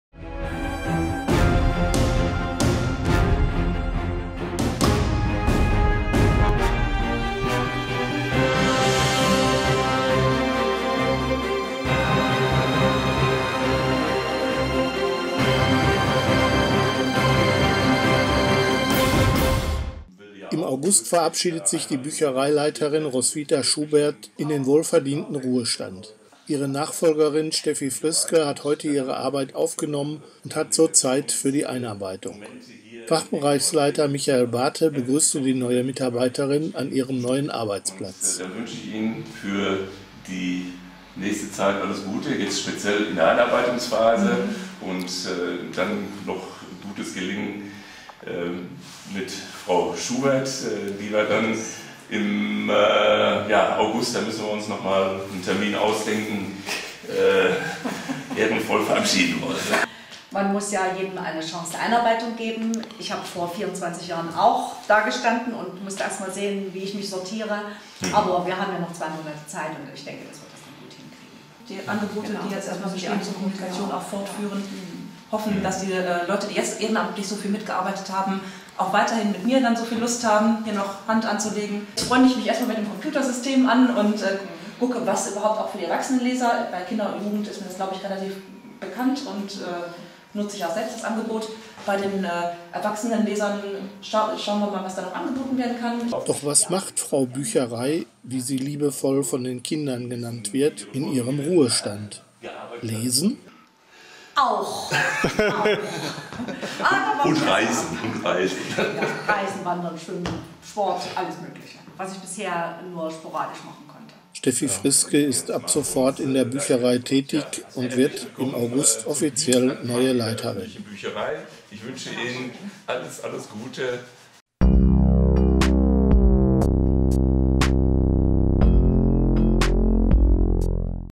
Steffi Friske wird Büchereileiterin (Balve)
Im August verabschiedet sich die Büchereileiterin Roswitha Schubert in den wohlverdienten Ruhestand. Ihre Nachfolgerin Steffi Friske hat heute ihre Arbeit aufgenommen und hat so Zeit für die Einarbeitung.